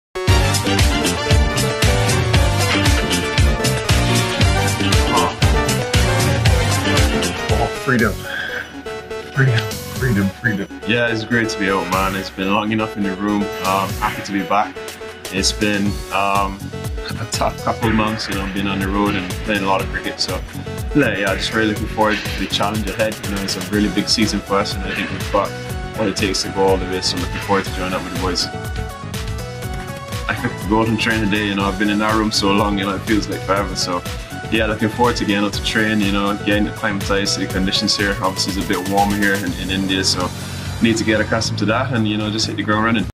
Out of quarantine ✅ Can't wait to get back ✅ Jason Holder has finished his quarantine 🙌
Out of quarantine ✅ Can't wait to get back ✅ @Jasonholder98 has finished his quarantine 🙌